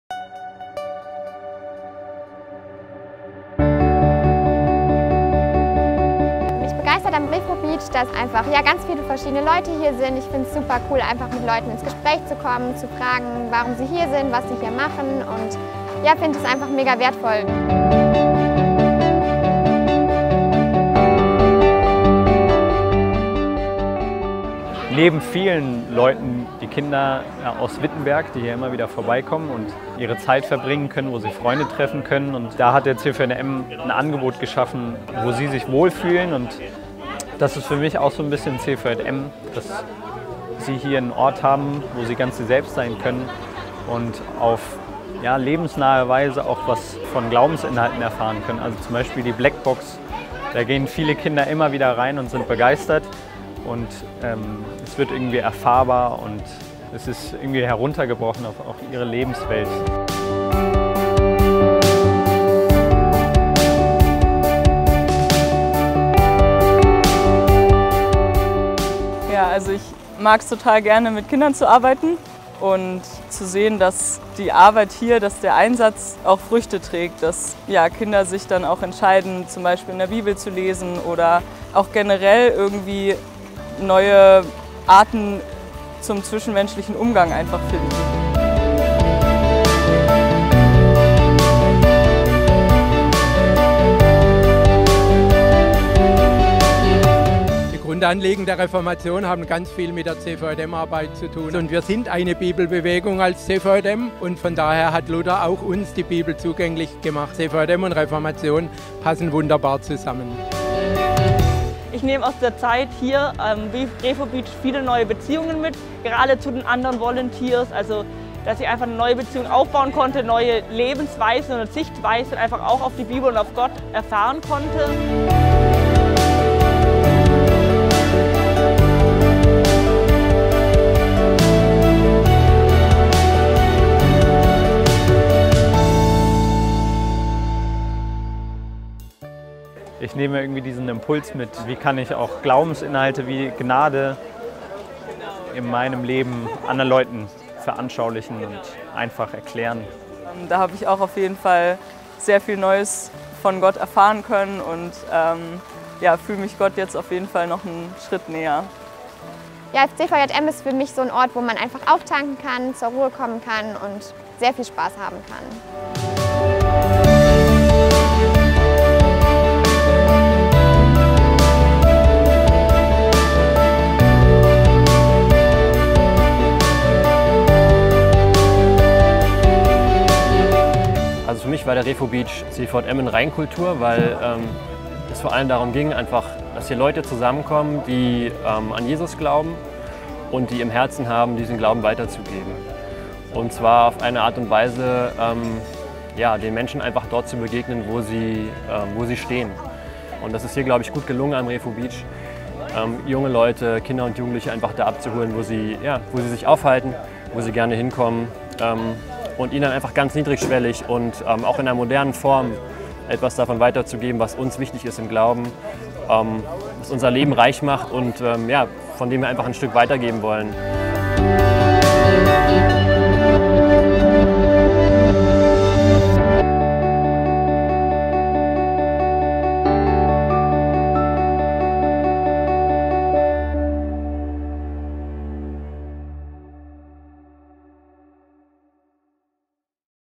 Refo.Beach in Wittenberg
Im Rahmen der "Weltausstellung Reformation" in der Lutherstadt Wittenberg hatte der Refobeach des CVJM von 20. Mai bis 10. September 2017 seine Pforten geöffnet.
Hier gibt es Impressionen vom Refo-Sommer auf dem schönsten Strand Wittenbergs: Ein Treffpunkt von jungen Menschen für junge Menschen.